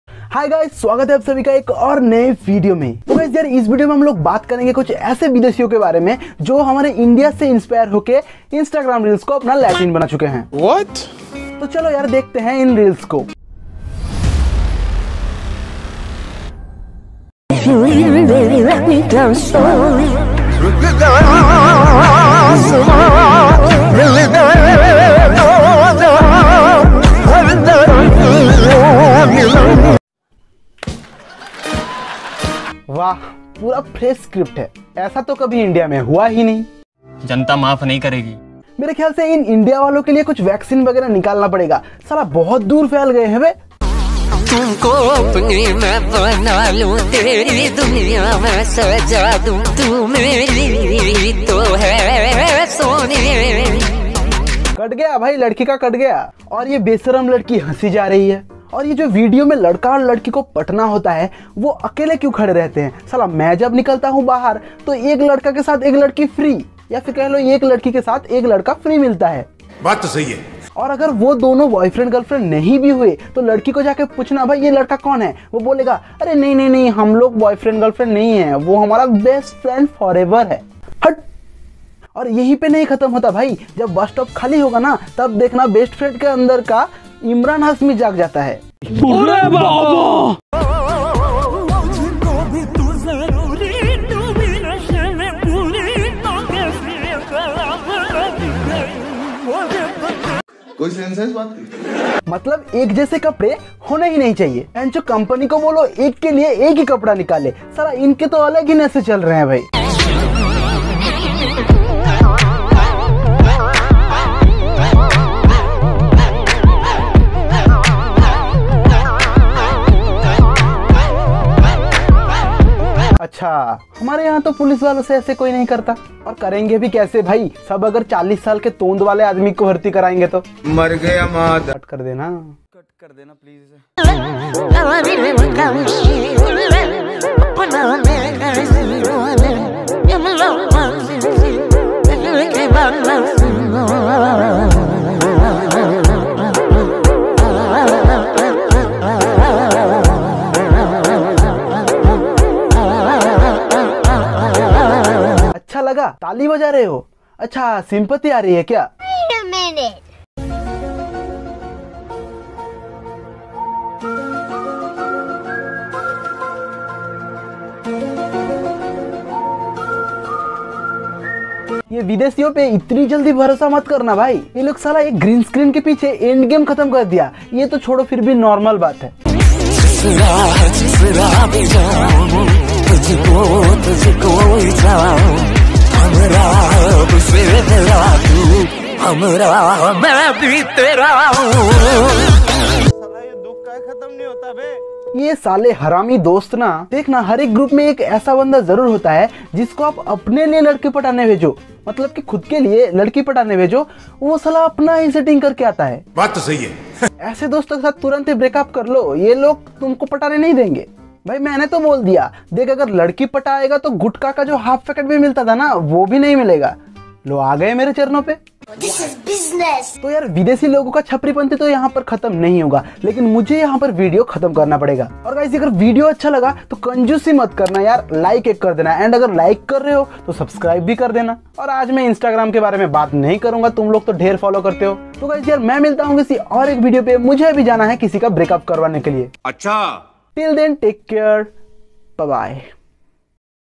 Proud of You INDIAN Chaprio || Bideshio Or Kuch Nahi Mila Copy Karneko
Hey Guys !  In this video I am roasting InstaGram reels Creat By Foreners .